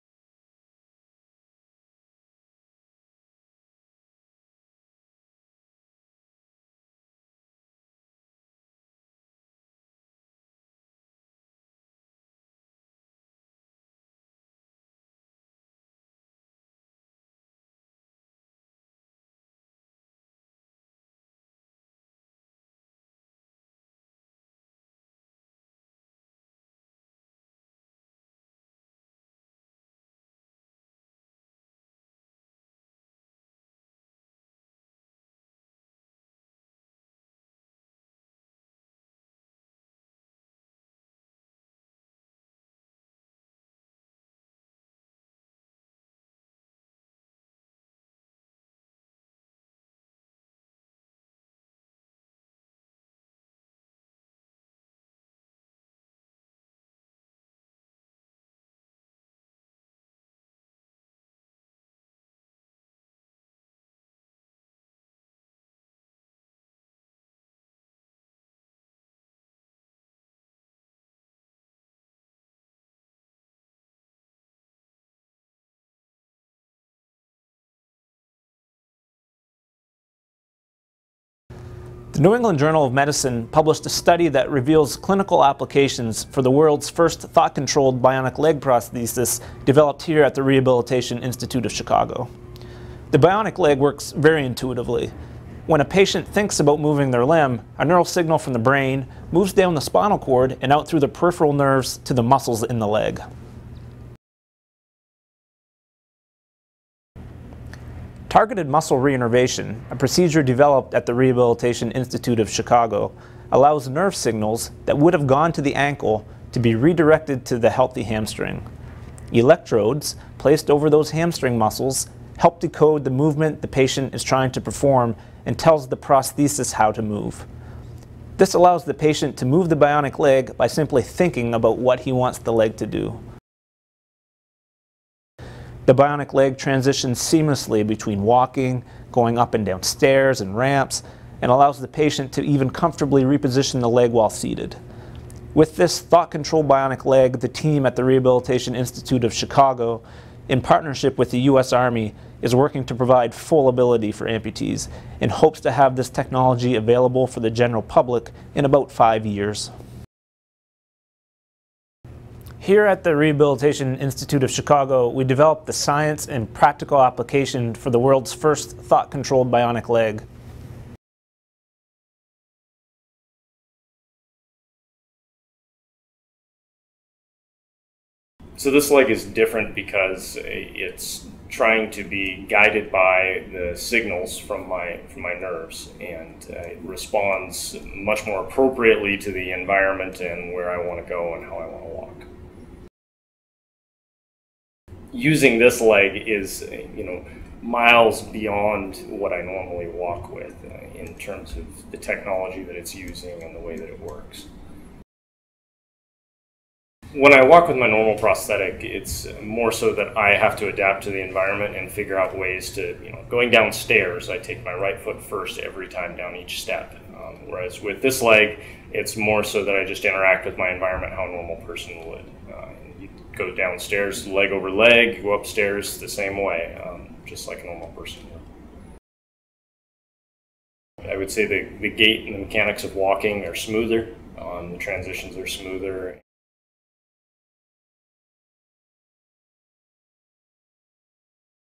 NEJM Case Study - Bionic Leg (B-Roll) | Rehabilitation Institute of Chicago
B-roll and sound bites for the New England Journal of Medicine Case Study, which describes clinical applications for the world's first thought-controlled leg, unveiled by Rehabilitation Institute of Chicago